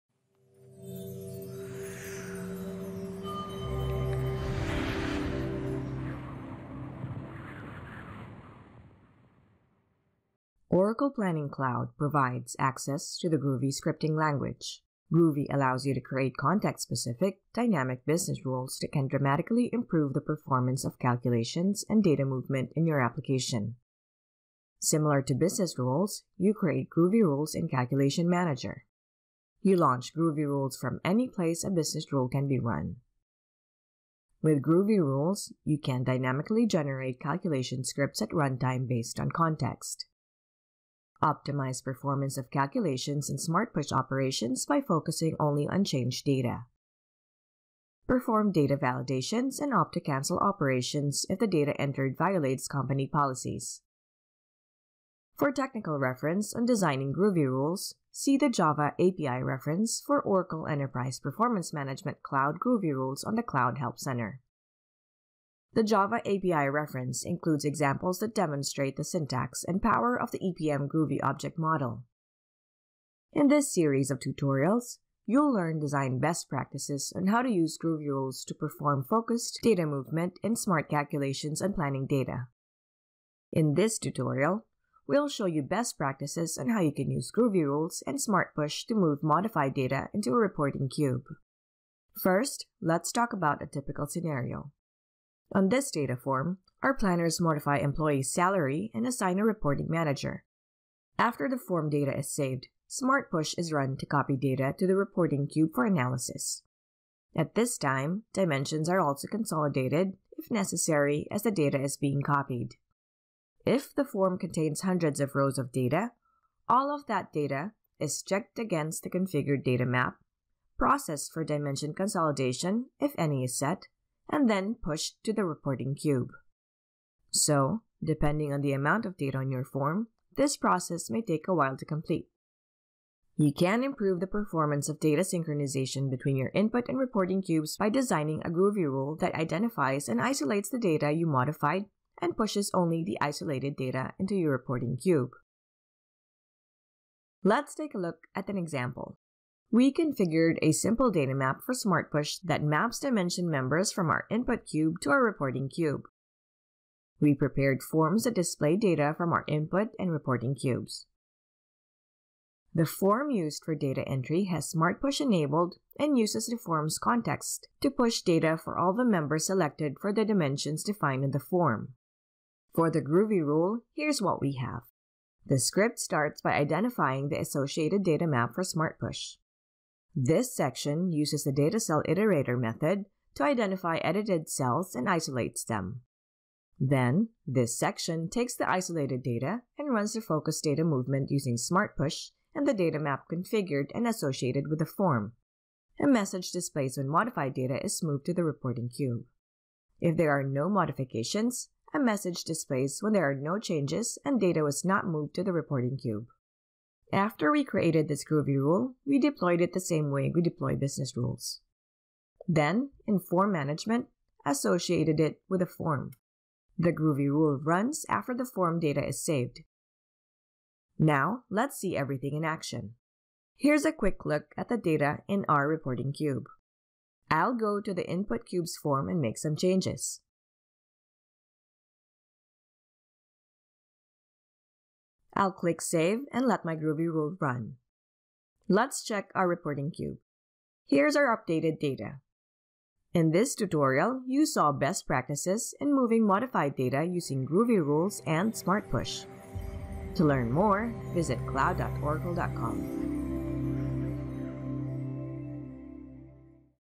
Moving Modified Data Using Groovy Rules and Smart Push
In this tutorial, you learn best practices in moving modified data using Groovy Rules and Smart Push. You can improve the performance of data synchronization between cubes by designing a Groovy rule that identifies and isolates the data you modified, and pushes only the isolated data into your target cube.

Enterprise Planning and Budgeting Cloud is now Planning, a business process in Oracle EPM Cloud